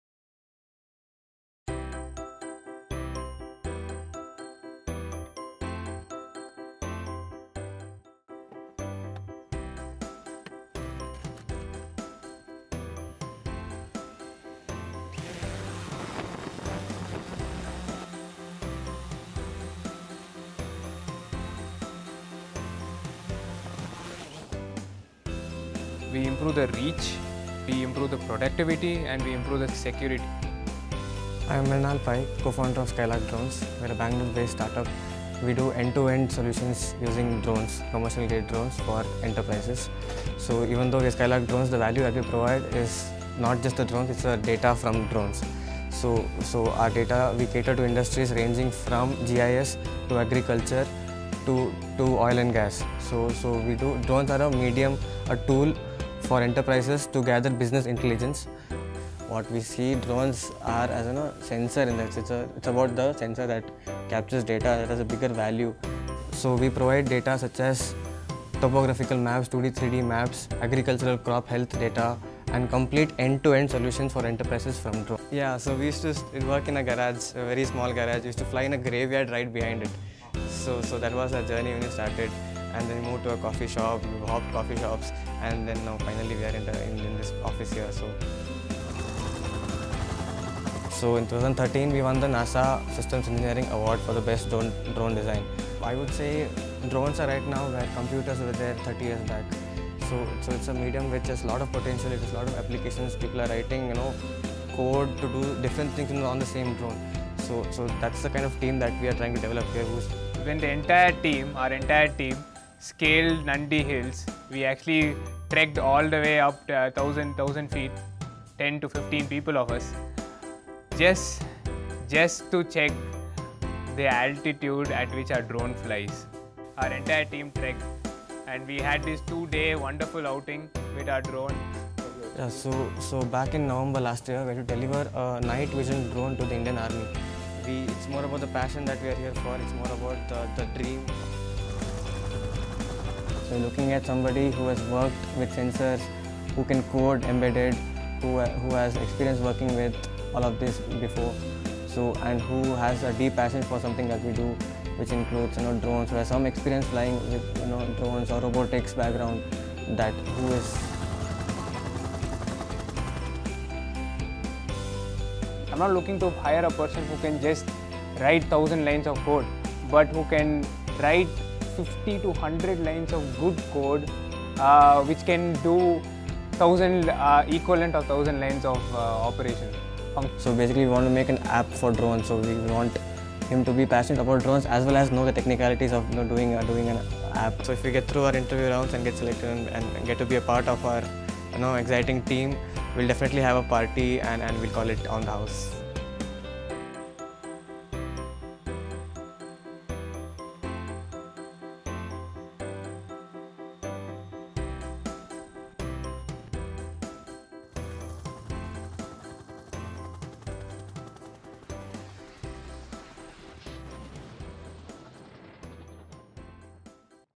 Skylark Drones on SPOTWRKS
Mrinal Pai & Mughilan, Co-Founder of Skylark Drones speaking about how they have started to working upon drones during their college days and their journey so far. And the type of people they want in their team.